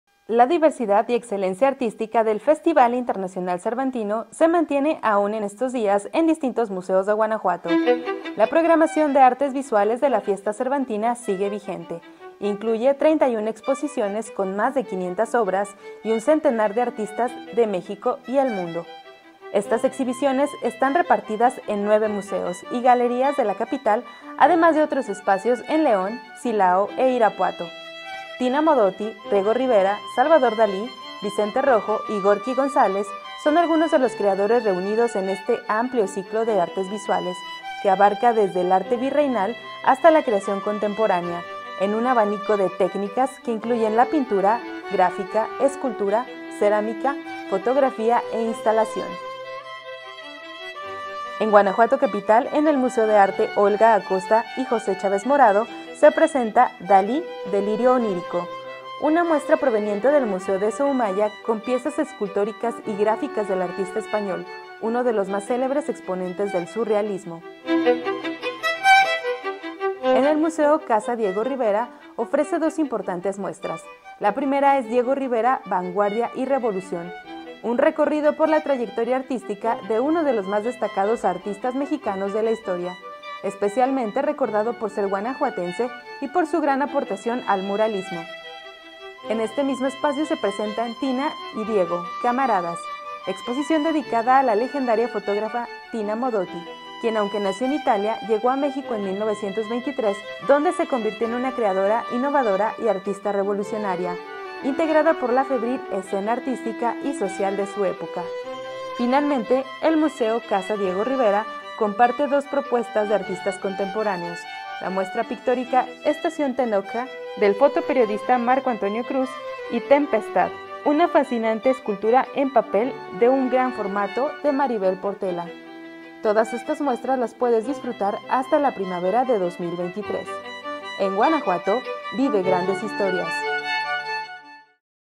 Visita las exhibiciones de artes visuales cervantinas | ZonaCultura🖼
Conoce más sobre las exhibiciones de artes visuales cervantinas que aún puedes visitar en distintos museos de Guanajuato.